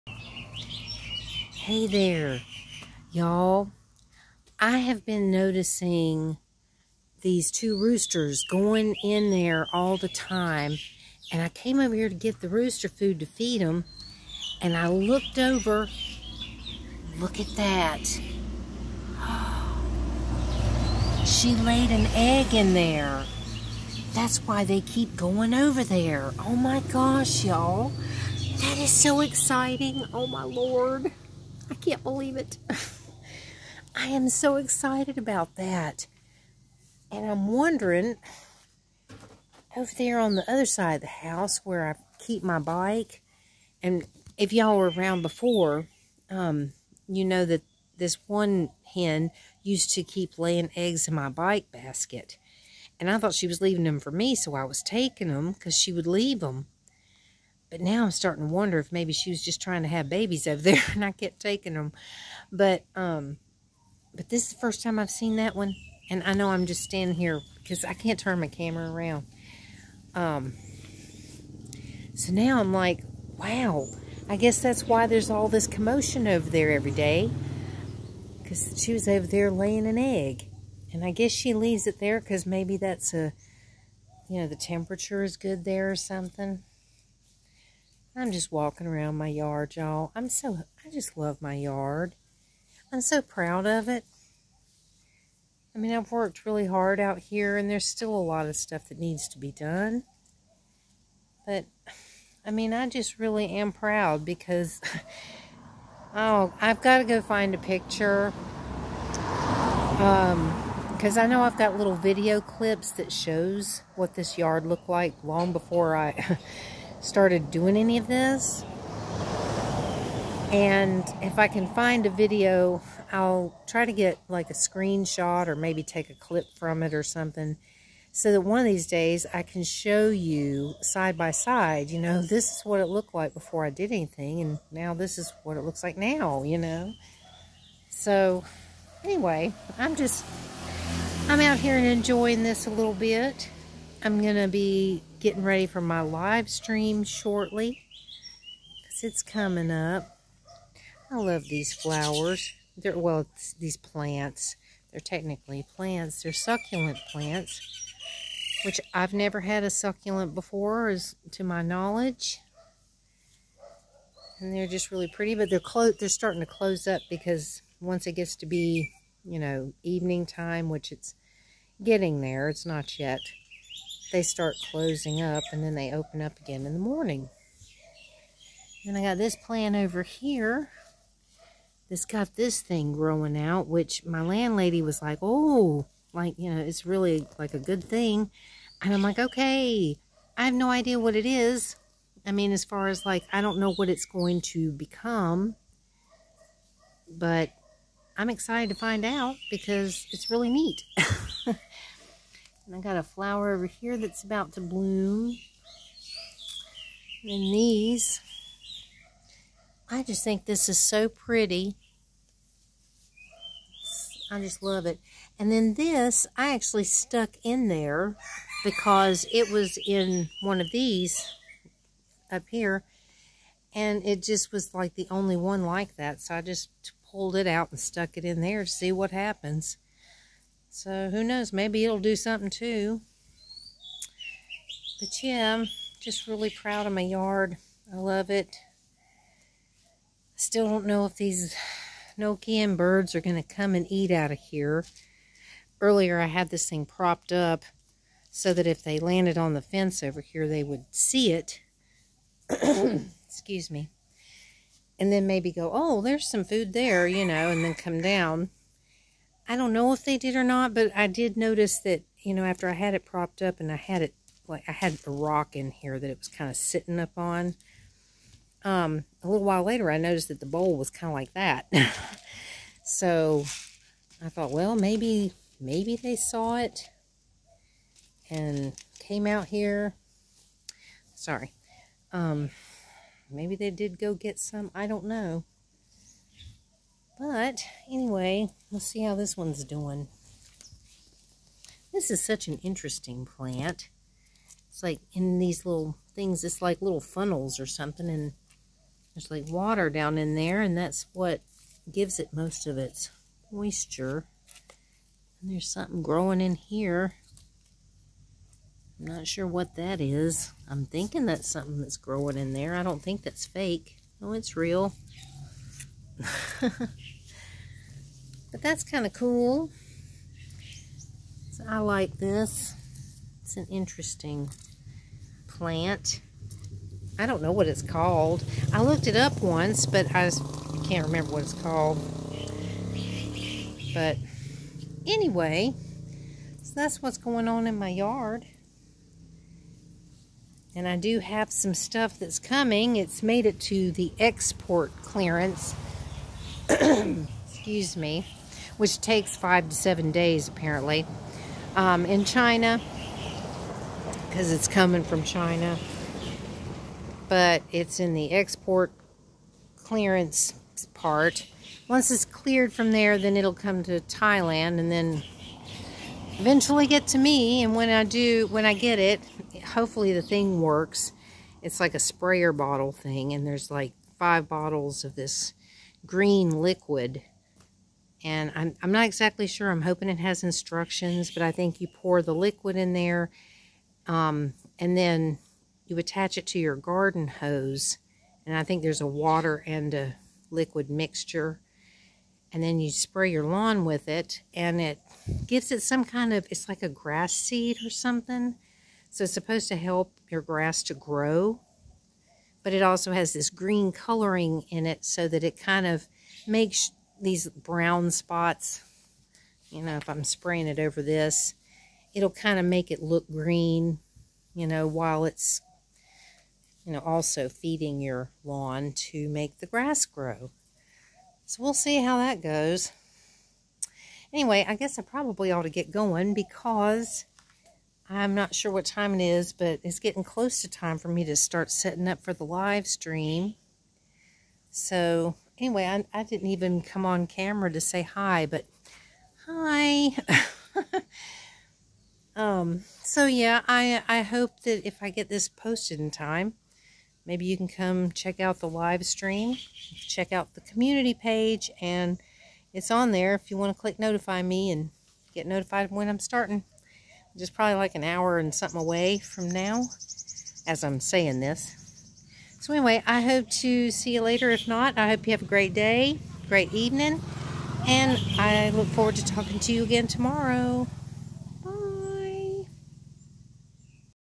A Walk In My Garden🌱- V183
Hey there! I made an unexpected find in my garden today! Let me show you! 😃